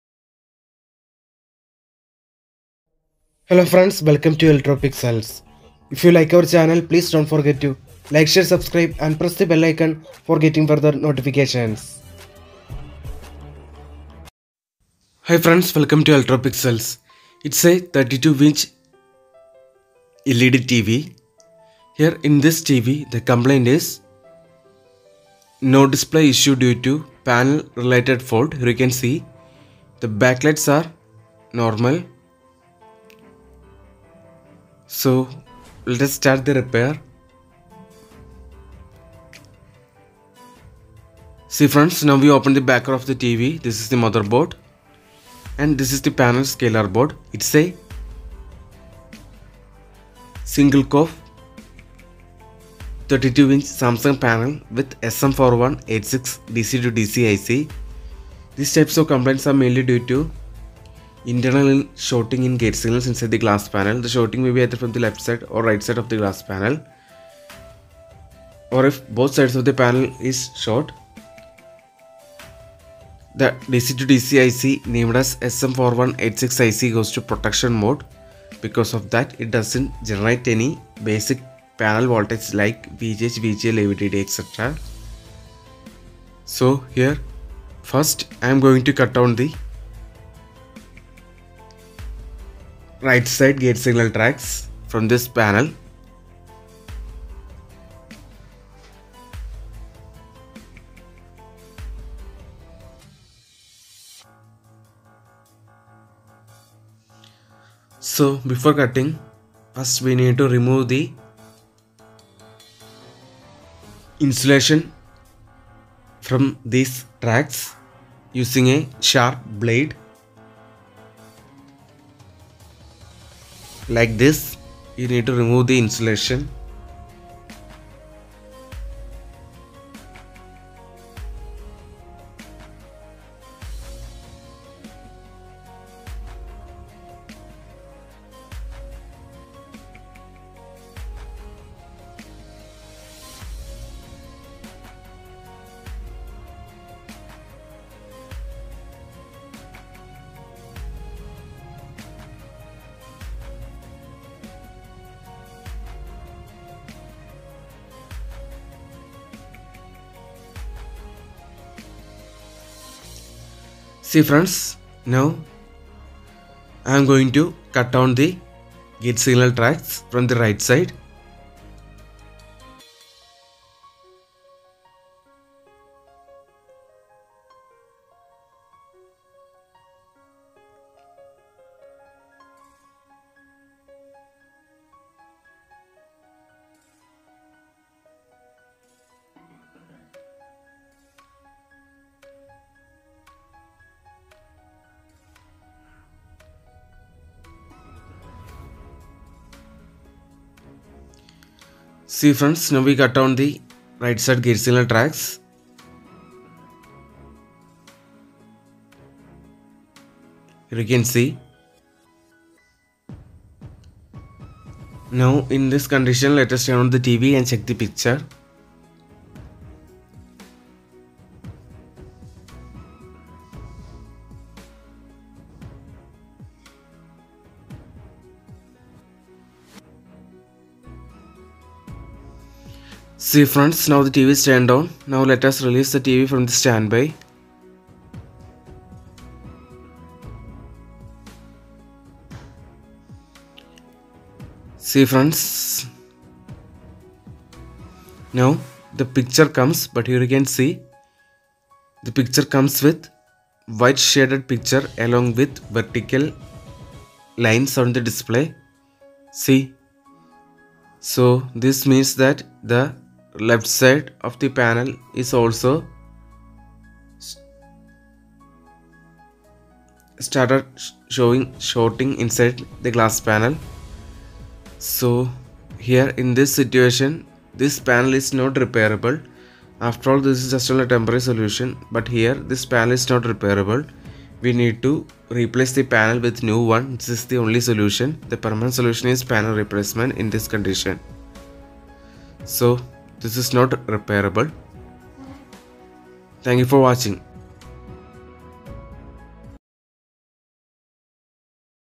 Led Tv panel short removal in Single Cof Panel with SM4186 Dc-Dc IC |Reparing Failed|Not Repairable
Hello friends today in this video, I will show you how to repair 32 inch led tv with blank screen Backlight is ok. In this video The modification process is explained in details.Our video is in English language so every body can understand easily.